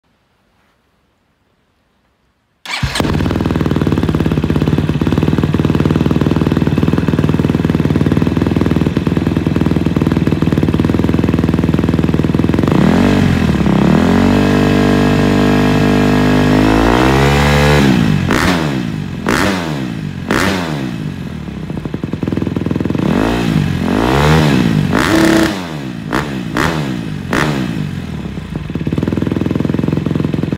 KTM 250 Pro Circuit T6 exhaust Start and Revs
Best sounding 4 stroke exhaust

Start and rev limiter pro circuit T6 full system on my 2019 Ktm 250xc-f

No, my bike is not smoking. The pipe is new and is burning the oils and coatings off.